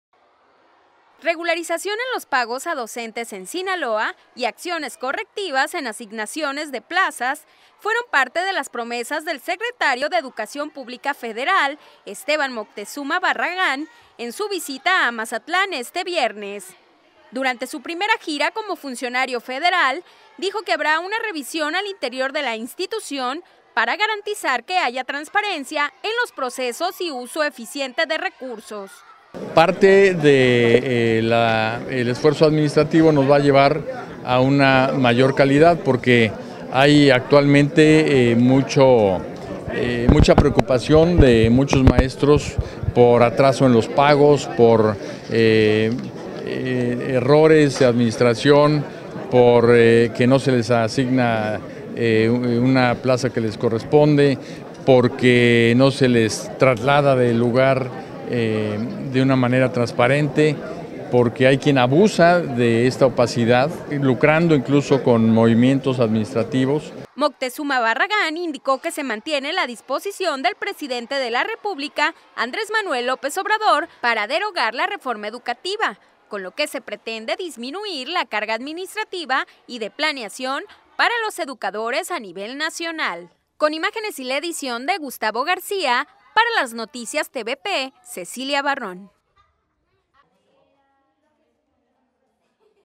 Prometen regularización en pagos a docentes en Sinaloa
En su visita a Mazatlán, el titular de la SEP federal, Esteban Moctezuma Barragán asegura que habrá inspección al área administrativa para garantizar transparencia y uso eficiente de recursos